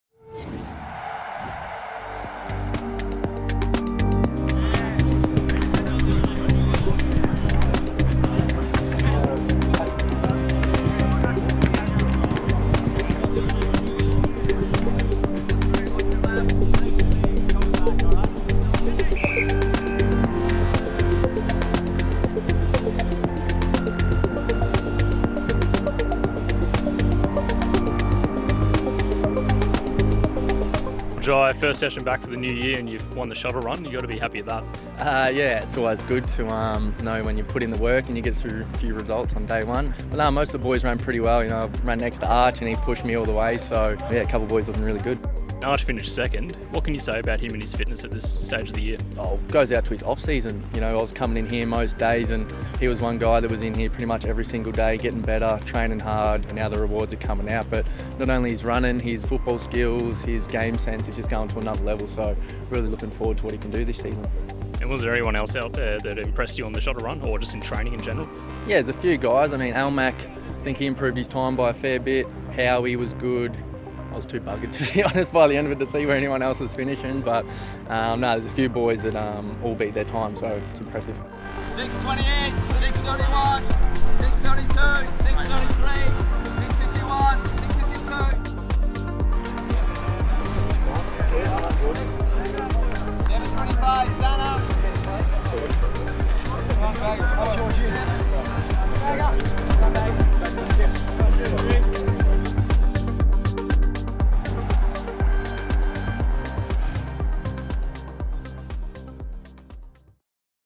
Roos return for first session of 2023 (January 9, 2023)
North Media speaks to midfielder Jy Simpkin after he took out the 1.8km shuttle run time trial as the players returned from their pre-season break.